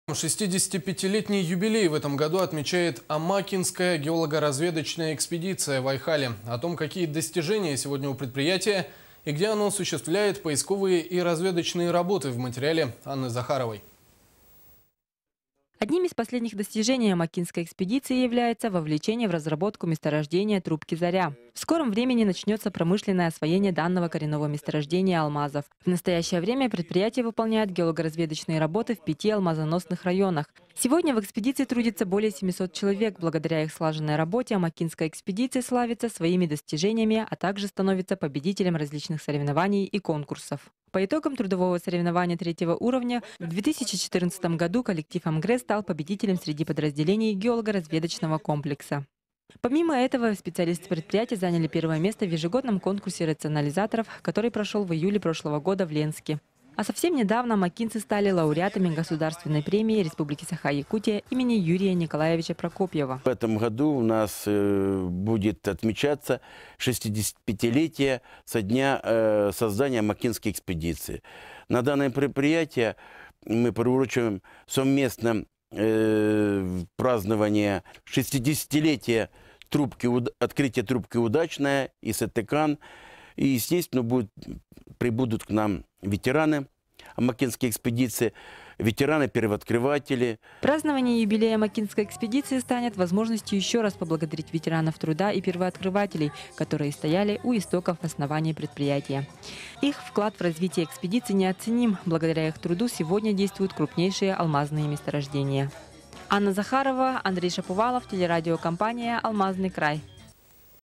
Амакинская геологоразведочная экспедиция отмечает 65-летие
65-летний юбилей в этом году отмечает Амакинская геологоразведочная экспедиция в Айхале. О том, какие достижения сегодня у предприятия, и где оно осуществляет поисковые и разведочные работы, смотрите в следующем материале.